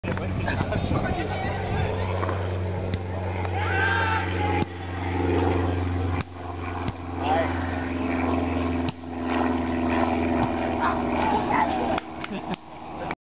CVSSC Volleyball Championship - Clip 7
The Charlottesville Sports and Social Club 2008 Spring volleyball championship set between Net Worth and The Sand Penguins.

Net Worth - Spring 2008 Volleyball Champions!